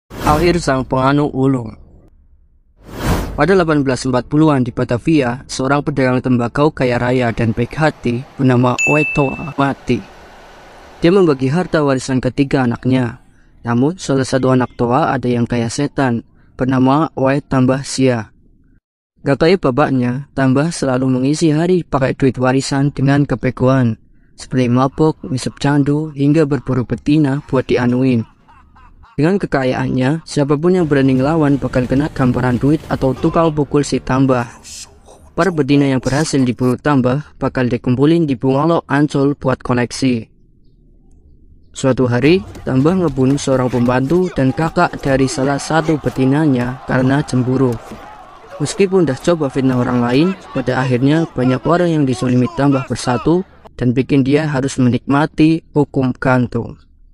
Akhir Tragis si Tukang Nganu
Sc:
tirto.id | Oei Tambah Sia, Playboy Batavia yang Ujung Hidupnya Tragis

detik.com | Akhir Hidup Playboy Oei Tambah Sia: Digantung di Balai Kota Batavia

goodnewsfromindonesia.id | Kisah Oei Tambah Sia, Playboy dari Batavia yang Berakhir di Tiang Gantung

voi.id | Oey Tambah Sia: Playboy Tionghoa di Batavia yang Mati Digantung karena Api Cemburu